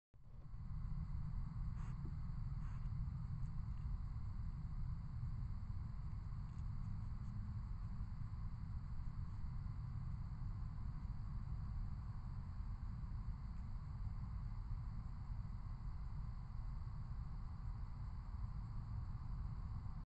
Don't look at wand.Look straight & what do you see?Read below.
I received a message by someone saying, that thing that looks like a spider web is a wand.From a Gray alien That's cloaking.cause of IR in camera.Web is not going across the screen,Watch what it does in all my videos.It's not a web..When you look straight that part starts to wave.Like something is there..Where is Scott carpenter..LOL..Did you see that Justin?